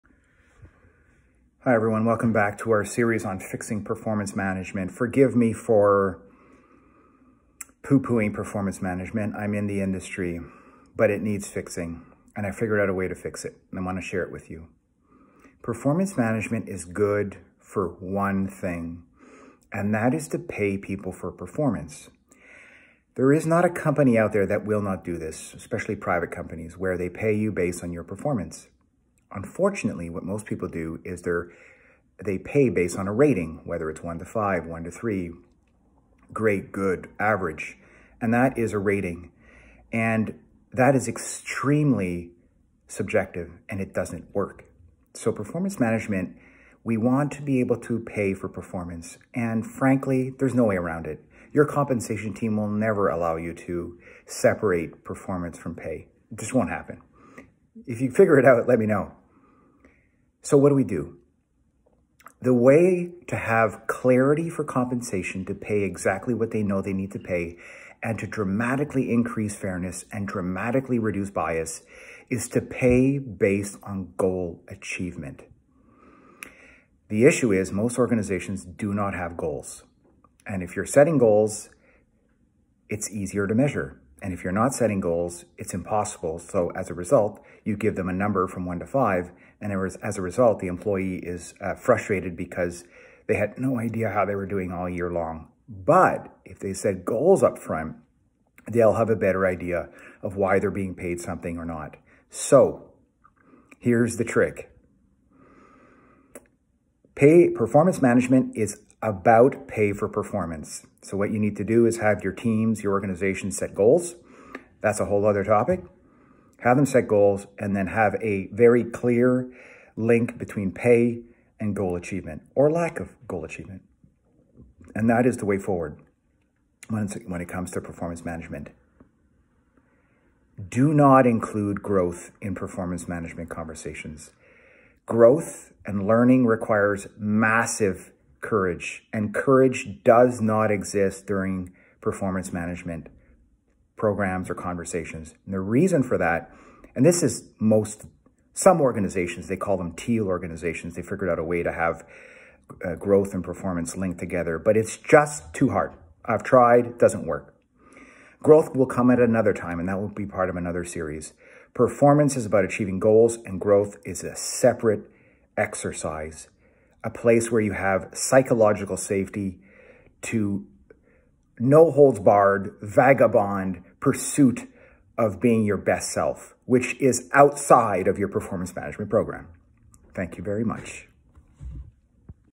The Only Benefit of Performance Management
While Performance Management has many problems and has proven ineffective in many organizations it clearly has the benefit of linking pay to performance. However, the pay-for-performance process must be done a certain way or else it will not work. Learn more in today's video.